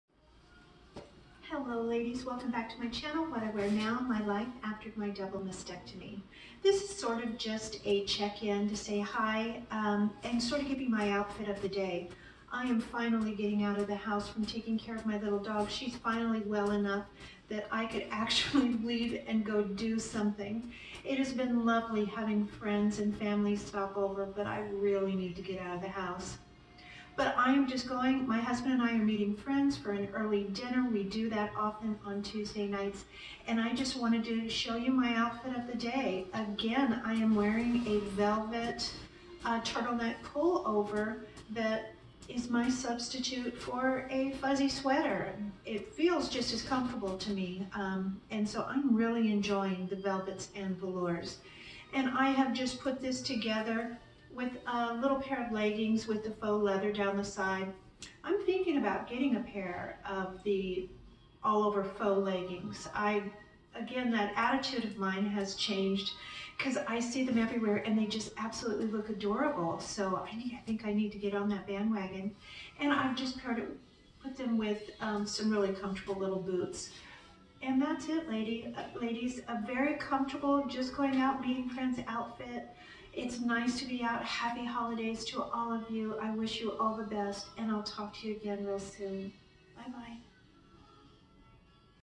Breast cancer: what I wear now. Another velvet top.
Hi Ladies!  I'm getting out of the house and wearing velvet again as a substitute for a sweater.  Very happy with this choice and I have found beautiful velvet at thrift stores.  This tunic was only a dollar! 😁